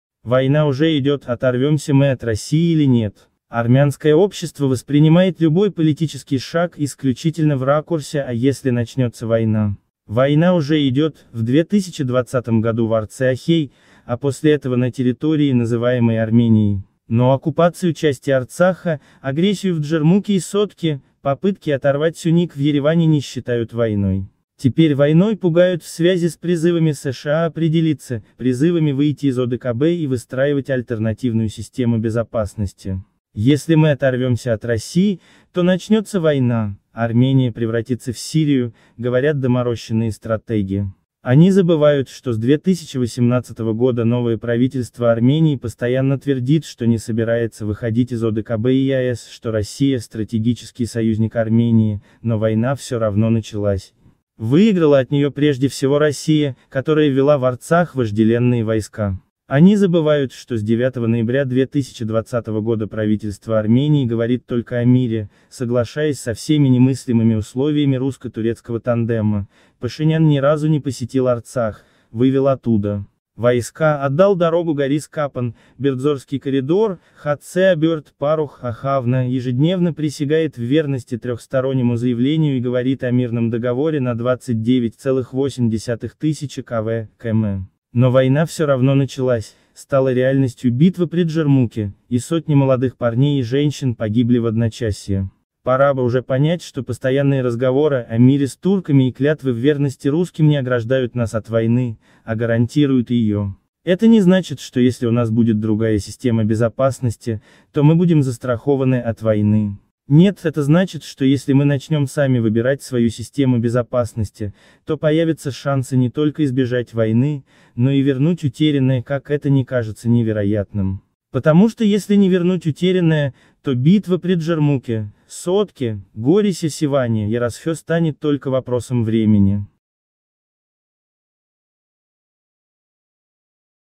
Война уже идет, оторвемся мы от России или нет
Армянское общество воспринимает любой политический шаг исключительно в ракурсе «а если начнется война». Война уже идет – в 2020 году в Арцахеь, а после этого на территории, называемой Арменией. Но оккупацию части Арцаха, агрессию в Джермуке и Сотке, попытки оторвать Сюник в Ереване не считают войной.

Теперь войной пугают в связи с призывами США определиться, призывами выйти из ОДКБ и выстраивать альтернативную систему безопасности. Если мы оторвемся от России, то начнется война, Армения превратится в Сирию, говорят доморощенные стратеги. Они забывают, что с 2018 года новое правительство Армении постоянно твердит, что не собирается выходить из ОДКБ и ЕАЭС, что Россия – стратегический союзник Армении, но война все равно началась, и выиграла от нее прежде всего Россия, которая ввела в Арцах вожделенные войска.

Они забывают, что с 9 ноября 2020 года правительство Армении говорит только о мире, соглашаясь со всеми немыслимыми условиями русско-турецкого тандема – Пашинян ни разу не посетил Арцах, вывел оттуда войска, отдал дорогу Горис-Капан, Бердзорский коридор, Хцаберд, Парух, Ахавно, ежедневно присягает в верности трехстороннему заявлению и говорит «о мирном договоре» на 29,8 тыс. кв. км. Но война все равно началась, стала реальностью “битва при Джермуке”, и сотни молодых парней и женщин погибли в одночасье.

Пора бы уже понять, что постоянные разговоры о мире с турками и клятвы в верности русским не ограждают нас от войны, а гарантируют ее. Это не значит, что если у нас будет другая система безопасности, то мы будем застрахованы от войны. Нет, это значит, что если мы начнем сами выбирать свою систему безопасности, то появятся шансы не только избежать войны, но и вернуть утерянное, как это ни кажется невероятным. Потому что если не вернуть утерянное, то битва при Джермуке, Сотке, Горисе, Севане, Ерасхе станет только вопросом
времени.
Армения
Арцах
Карабах
Азербайджан
Срочные новости

Экстренные новости
Новости дня
Վերջին լուրեր
Յութուբ լուրեր
Շտապ